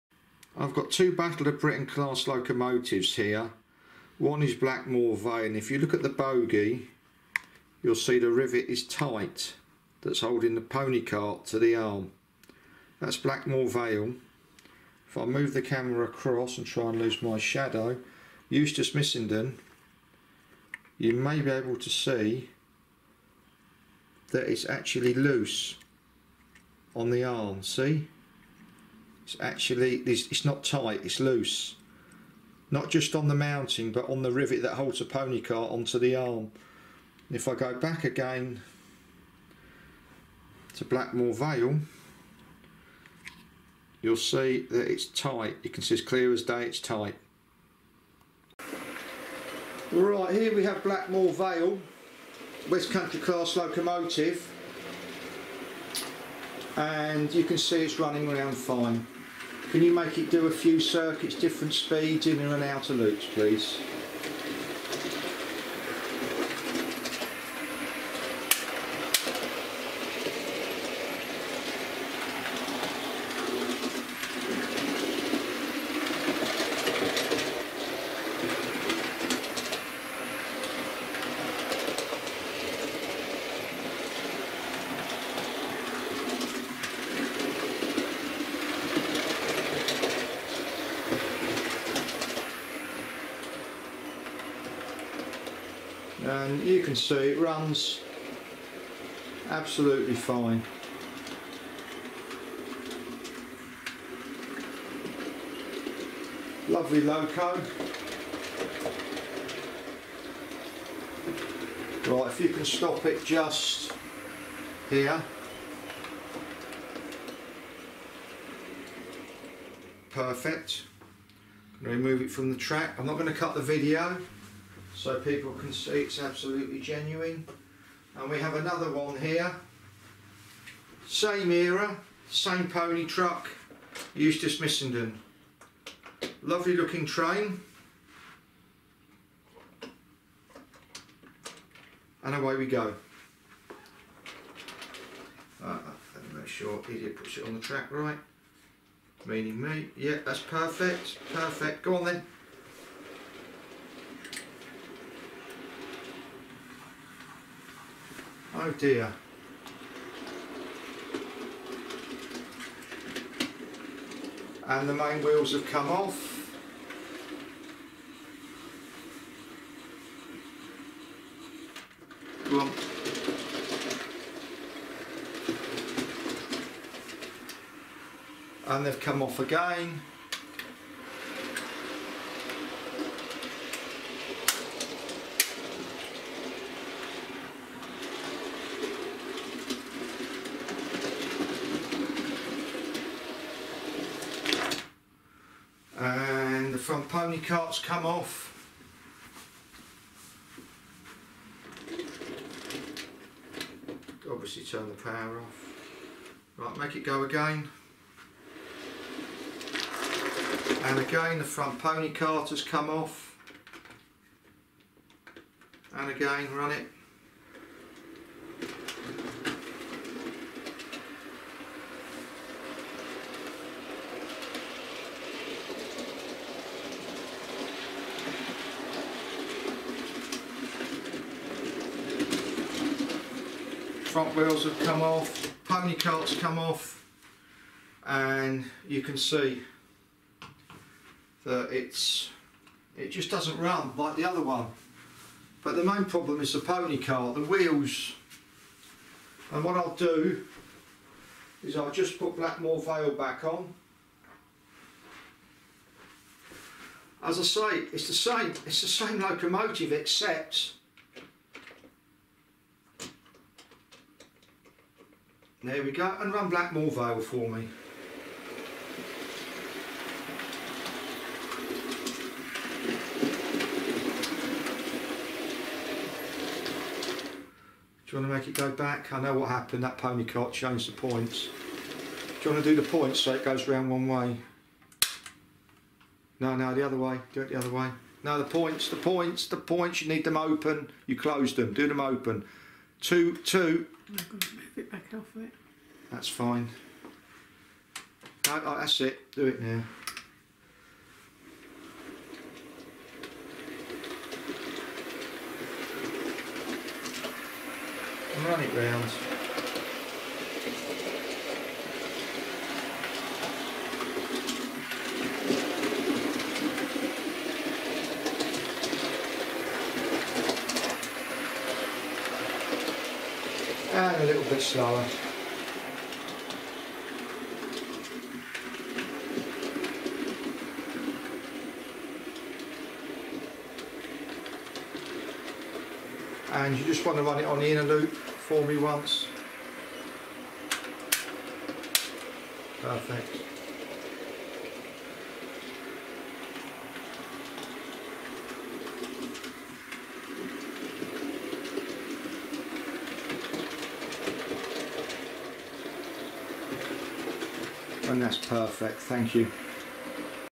Pony cart problem Hornby West Country/ Battle of Britain class locomotive
I have bought two R2692 locomotives Sir Eustice Missenden, they both have the same problem the front Pony cart cannot navigate points.  I also have Blackmoor Vale (R2219) that runs absolutely fine.  The problem with both R2692 locomotives is the rivet that holds the pony cart to the mounting arm is loose where as on R2219 it is tight.  If it were a screw fixing like on most locos you could put a small spring in to take up the slack but with a rivet your options are not there.  A great shame as it is a lovely looking loco